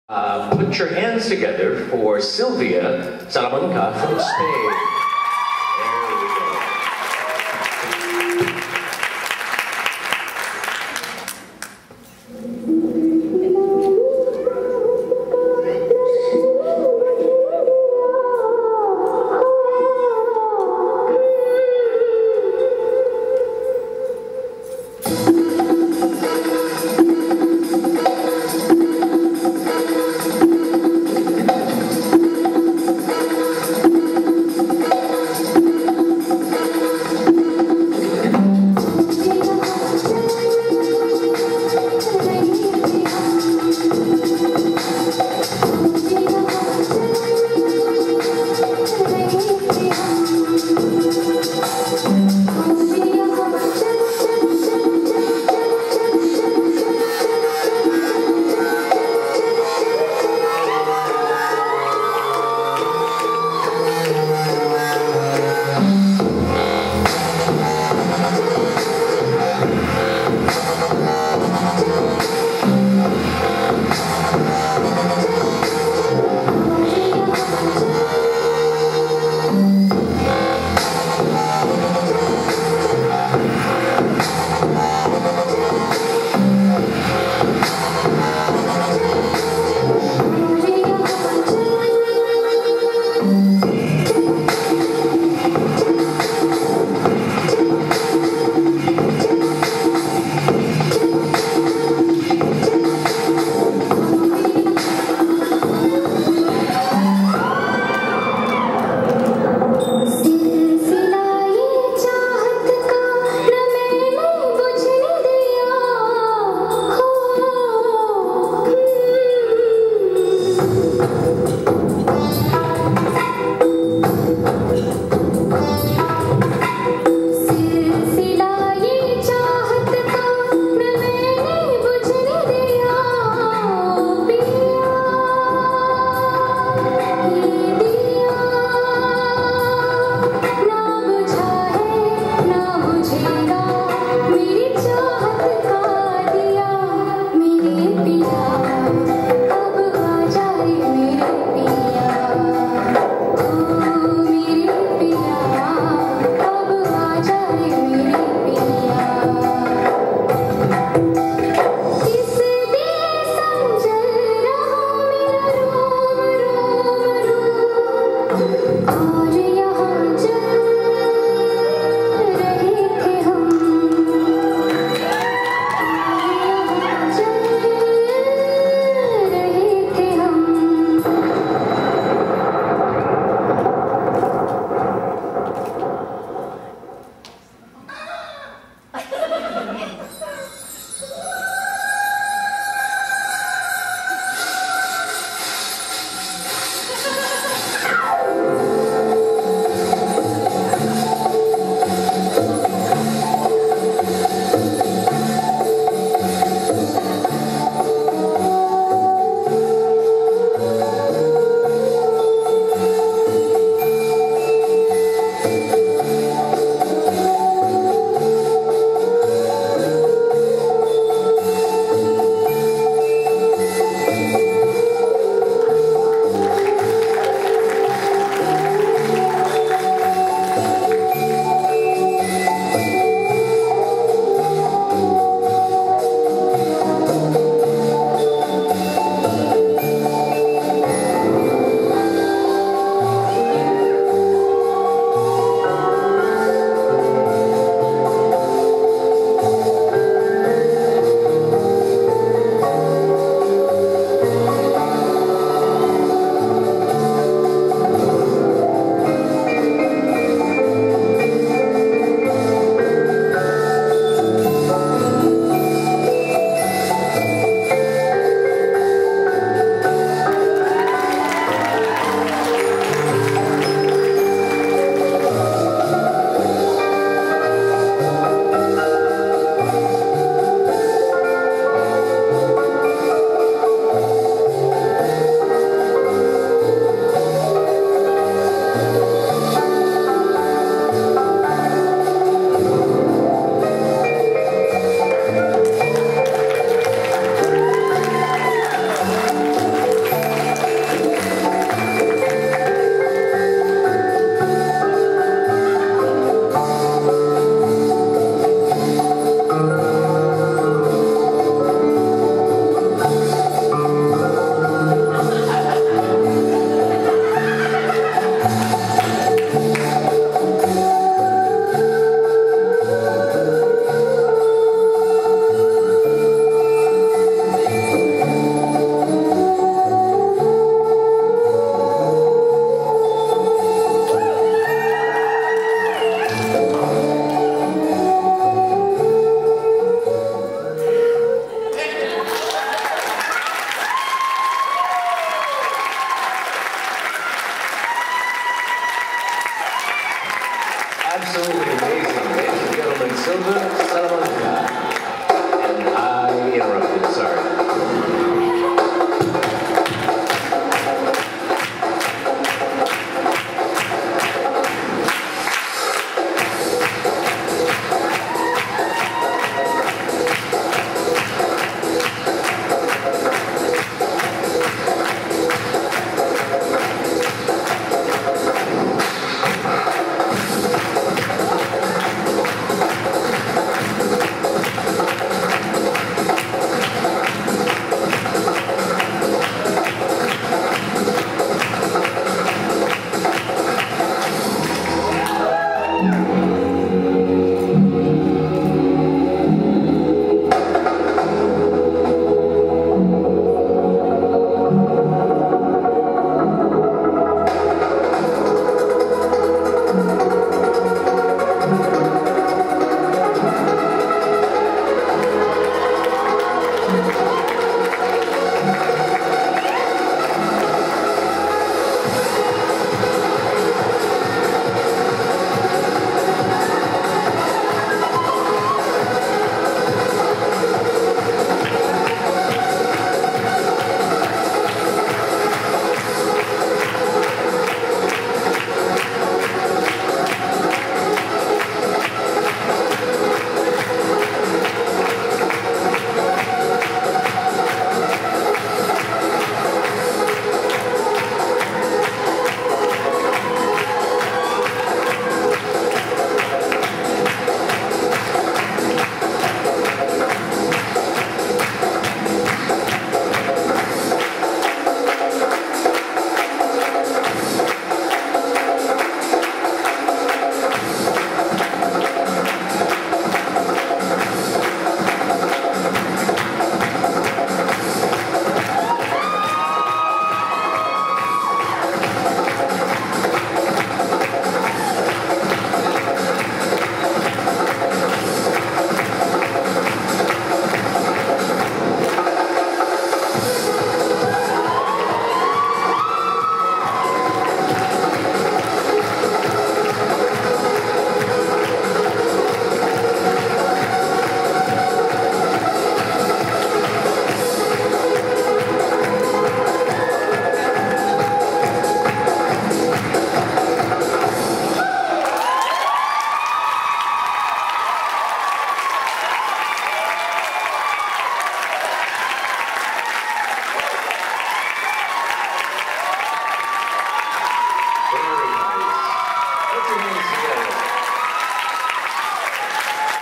Silvia Salamanca at 2011 Belly Dance Masters
Silvia Salamanca... WOW!!!... from 2011 Bellies for Babies in Orlando produced by Stellar Advantage (stellaradvantage.com)